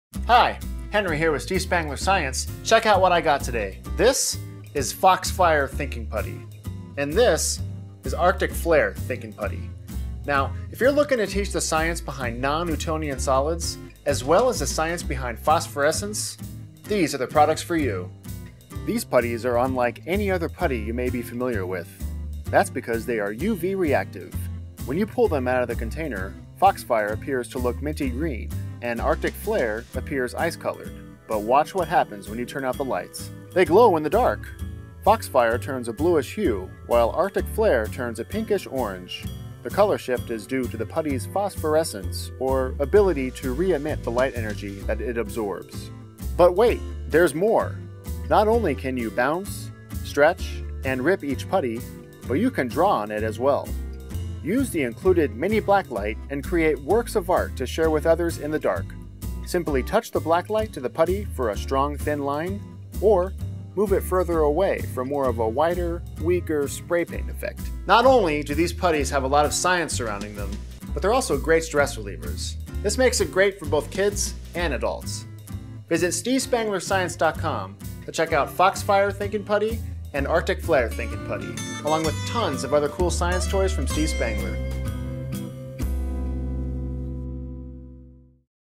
Foxfire Thinking Putty/Arctic Flare Thinking Putty
When you want your putty to do more than be an awesome non-Newtonian thinking aid, then take a look at Foxfire Thinking Putty and Arctic Flare Thinking Putty. These special, shimmering green and ice Thinking Puttys make for great stress relievers, but when you turn out the lights, they reveal their amazing secret: they glow in the dark!

Whether you charge these Thinking Puttys with normal lighting or use the included keychain black light to write on them, these phosphorescent puttys will blow your mind while keeping your stress levels down!